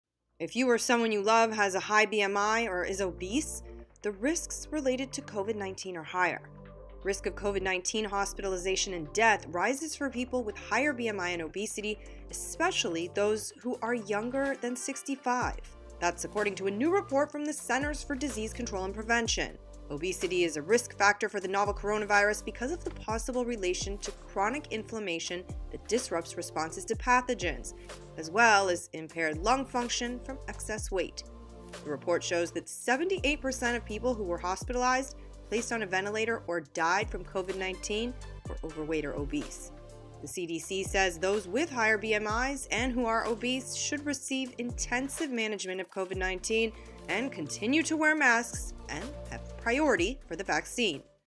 Obesity and High BMI are Covid-19 Risk Factors: CDC
The CDC says the risk of complications or death due to Covid-19 rises for those with high BMI or obesity. Veuer’s Elizabeth Keatinge has more.